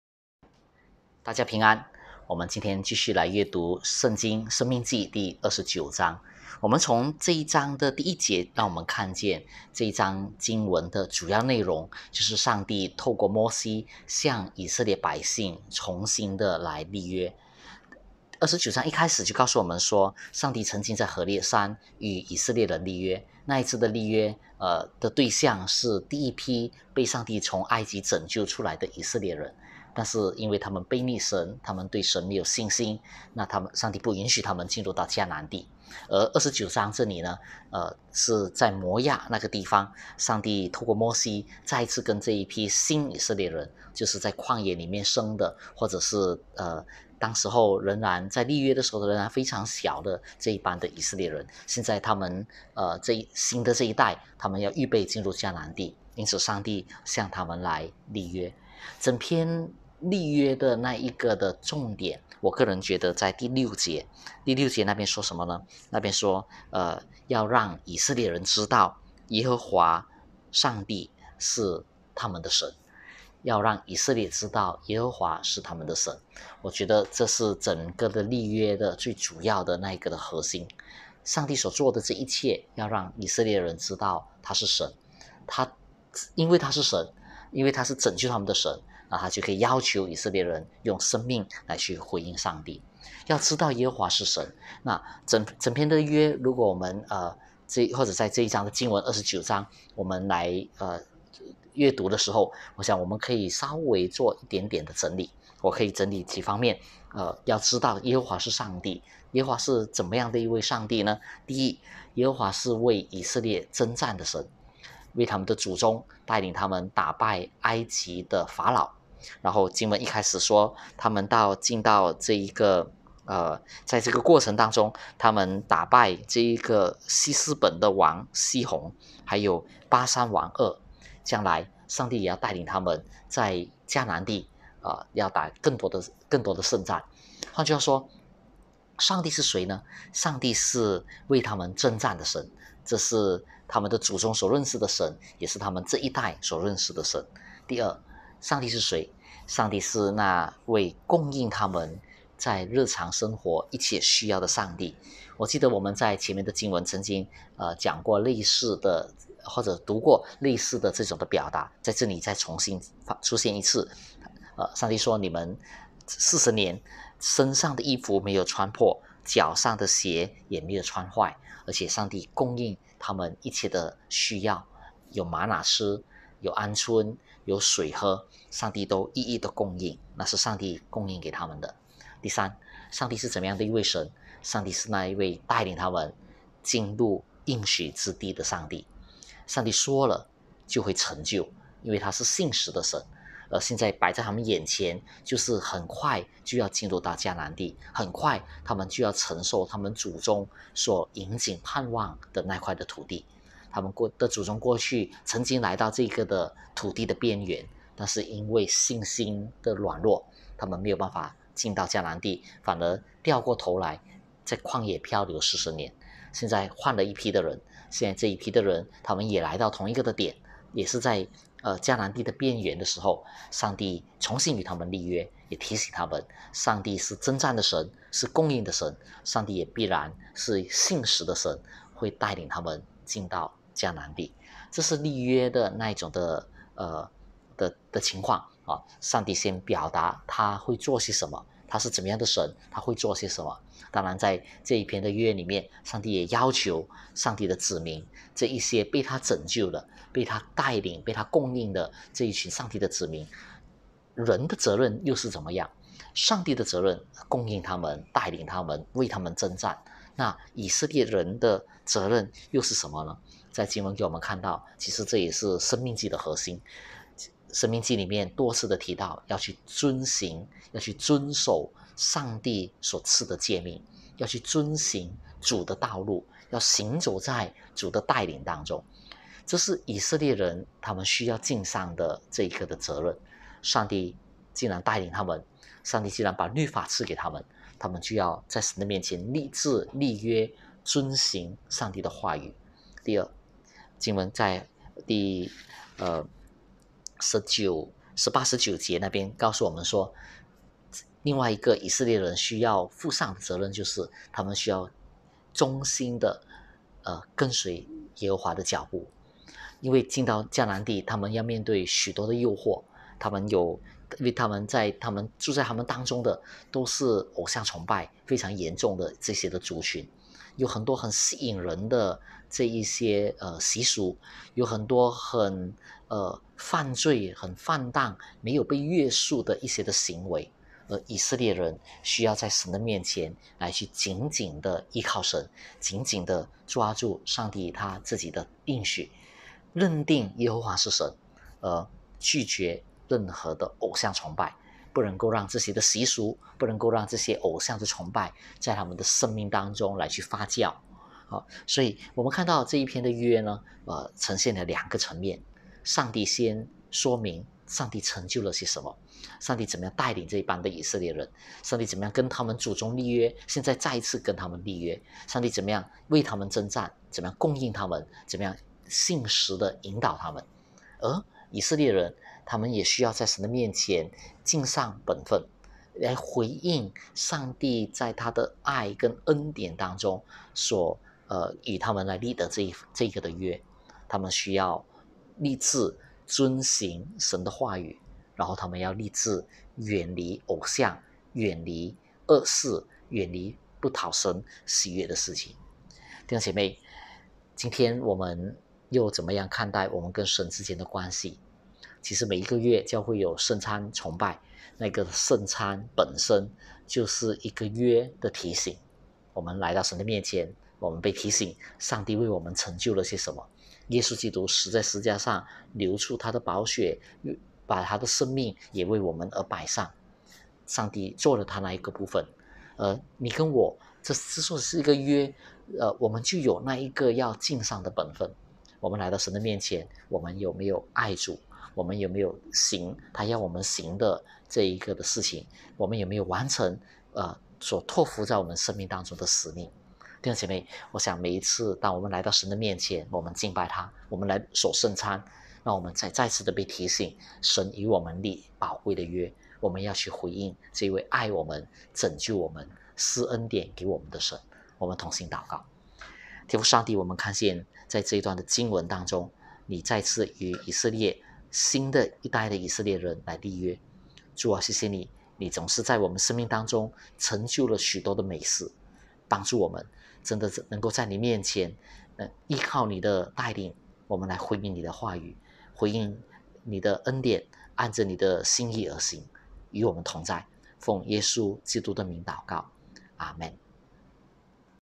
20200806 申命记29：1 by 汤鹏翔牧师
「再次立约」

上帝再次与以色列人立约，这不是新的约，而是旧约的重申，但当中绝大部分都是新时代的以色列人。整个立约的焦点在于，要让以色列认识耶和华是他们的上帝！

欢迎分享。